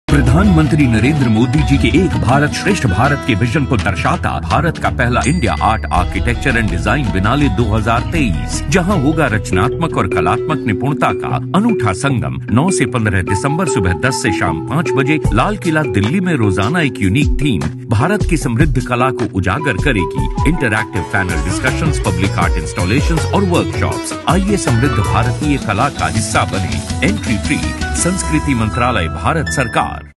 Visit the India Art, Architecture & Design Biennale 2023 now!
प्रधानमंत्री नरेंद्र मोदी जी के एक भारत श्रेष्ठ भारत के विजन को दर्शाता भारत का पहला इंडिया आर्ट, आर्किटेक्चर एंड डिज़ाइन बियनाले 2023, जहां होगा रचनात्मक और कलात्मक निपुणता का अनूठा संगम 9 से 15 दिसंबर सुबह 10 से शाम 5 बजे तक, लाल किला में आइए और समृद्ध भारत की कला का हिस्सा बनें।

📍Red Fort, New Delhi
🕐 10 am to 5 pm.
🗓️ 9th to 15th December, 2023